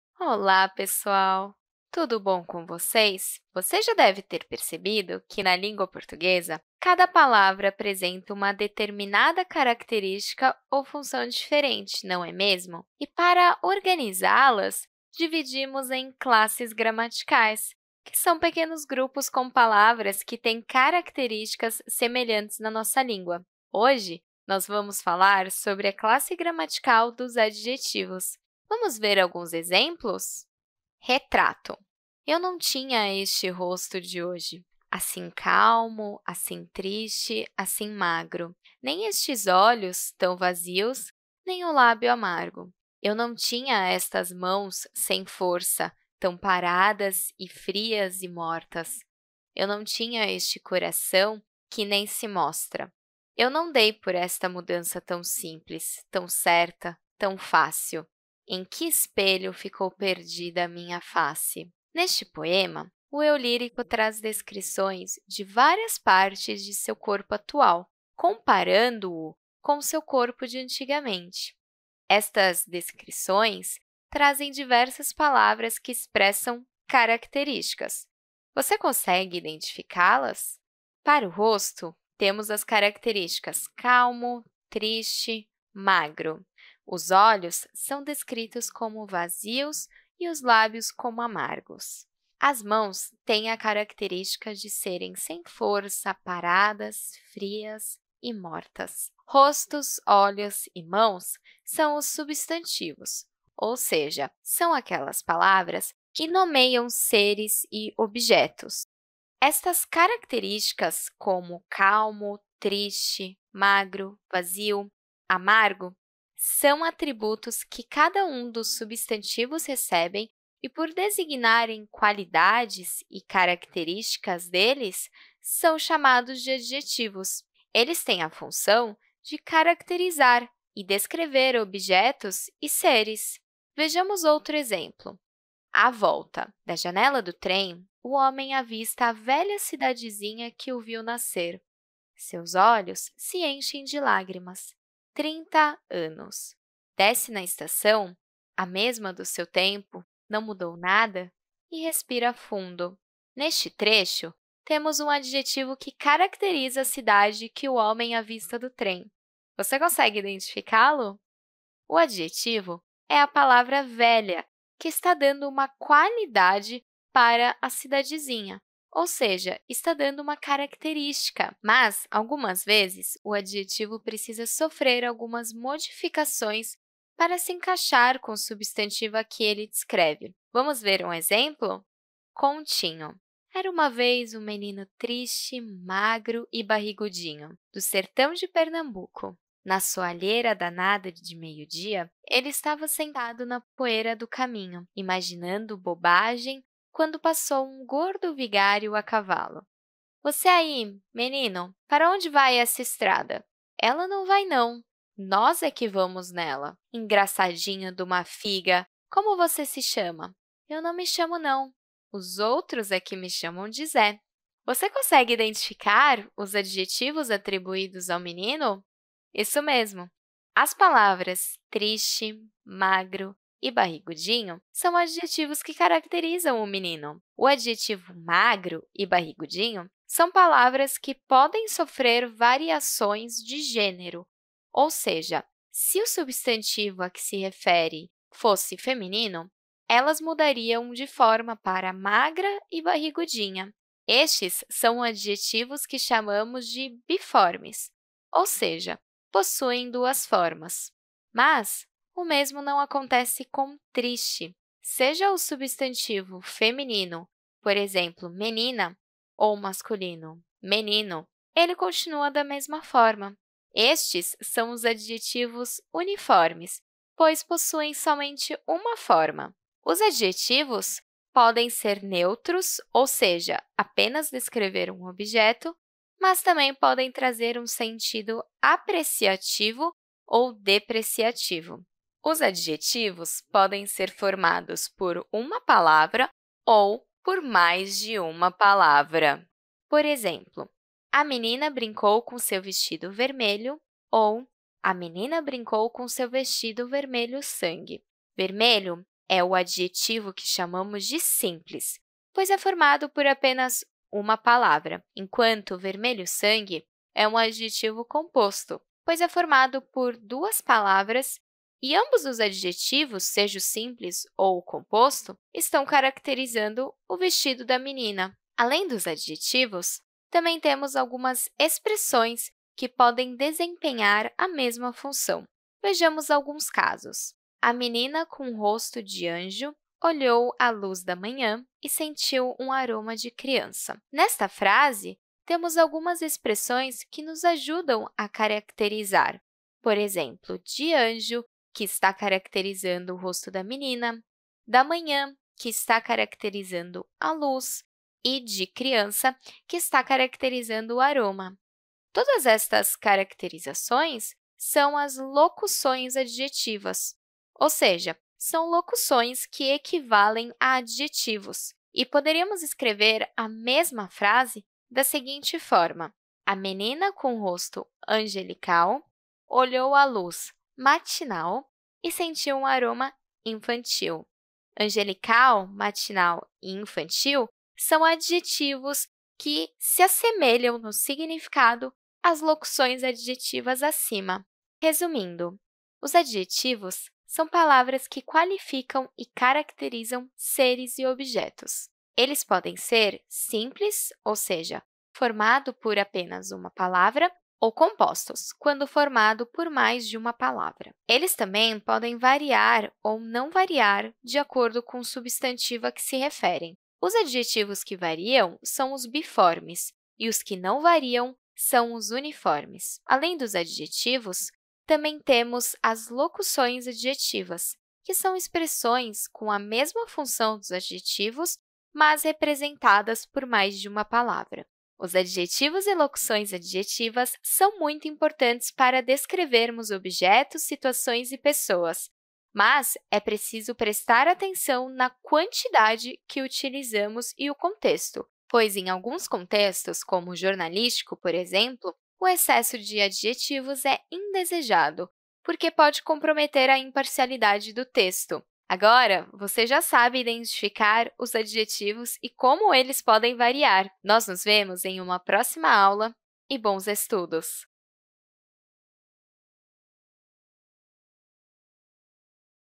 Classe gramatical: adjetivos
Nesta videoaula, apresentamos os adjetivos, abordando sua definição, função e exemplos. Também são apresentadas locuções adjetivas, além da classificação de adjetivos simples e compostos.